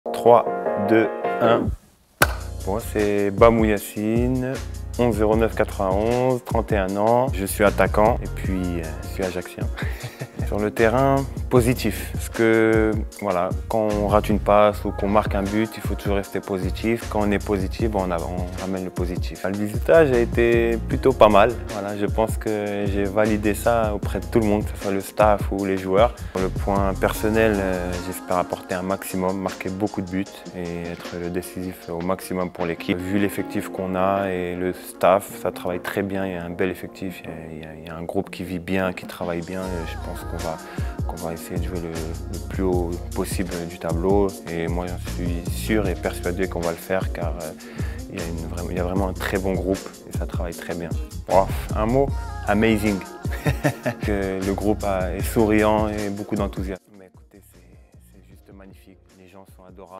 A la découverte de Yacine Bammou
Notre nouvel attaquant, Yacine Bammou, nous a livré ses premiers sentiments après la préparation estivale. Découvrez son interview en vidéo.